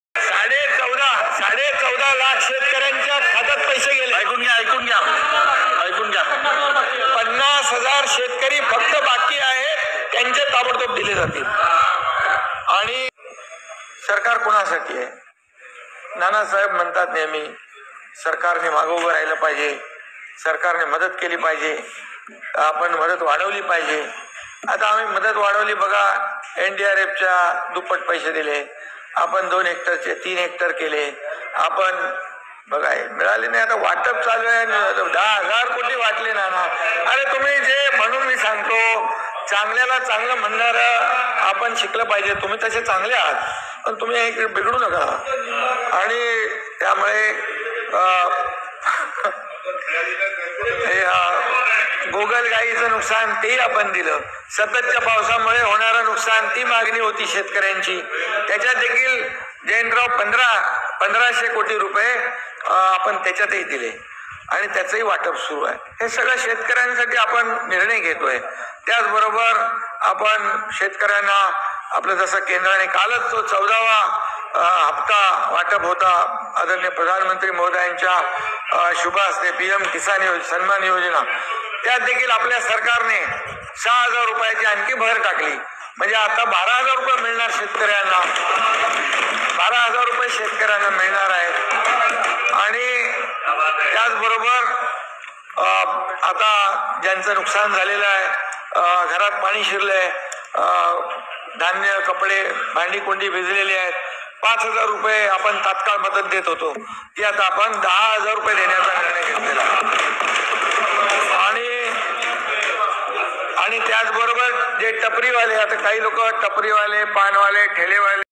अतिवृष्टी व गारपीटीचे पैसे शेतकर्यांच्या खात्यात जमा
अतिवृष्टी नुकसान भरपाई
गारपीट नुकसान भरपाई